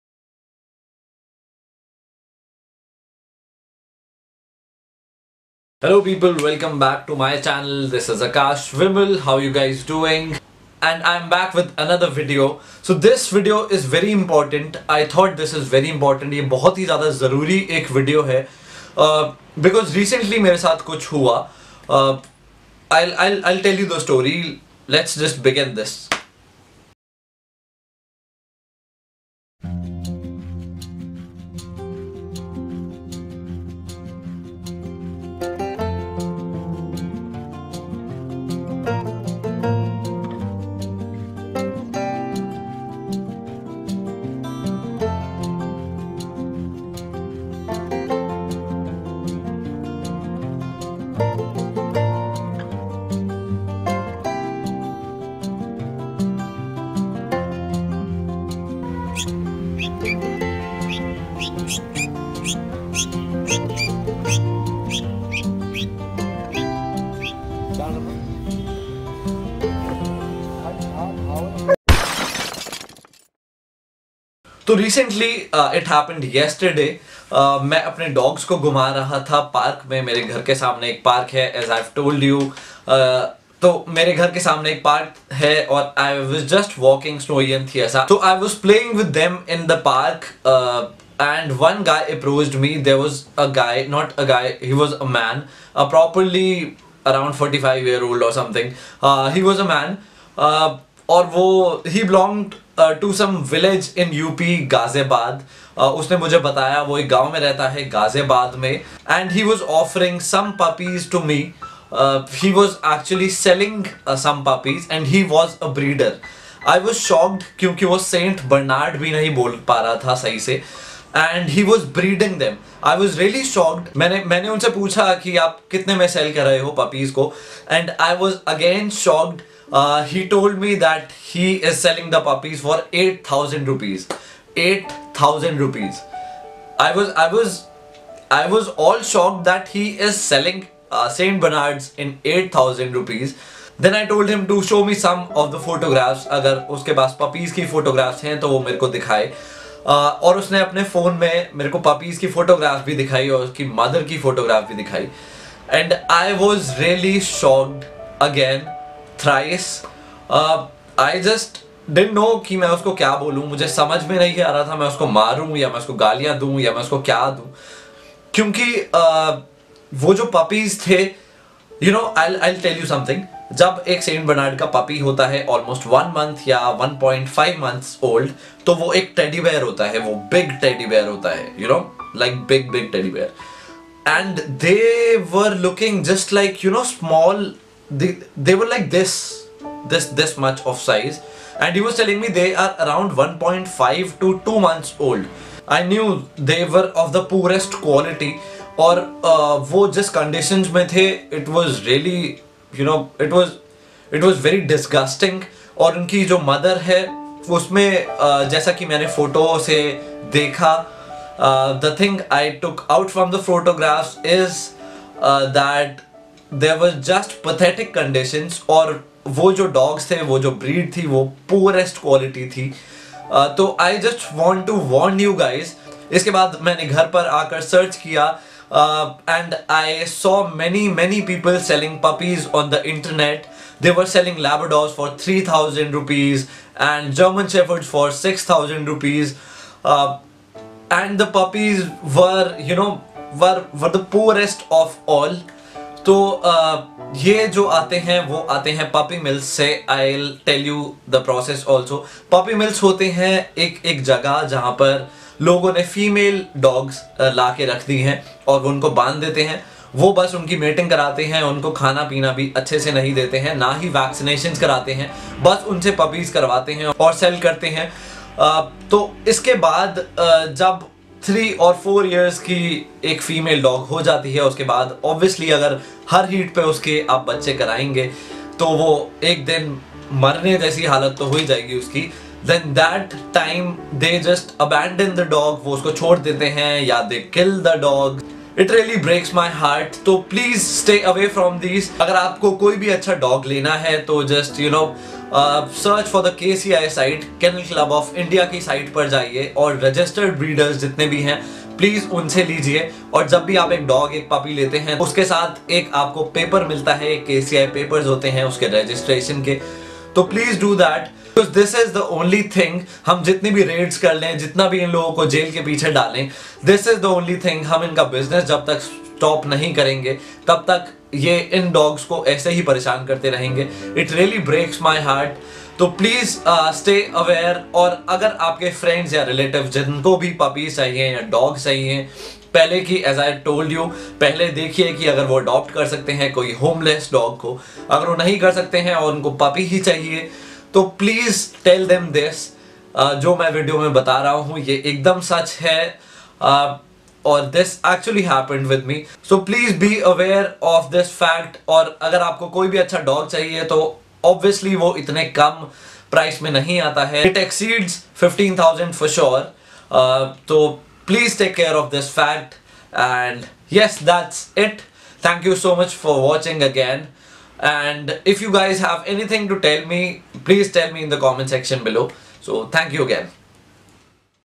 Saint Bernard puppy for 7,000Rs.? | Wildly Indian
People often look for cheapest for anything or everything they want. Recently I got to know about something very devastating. And this video is all about that.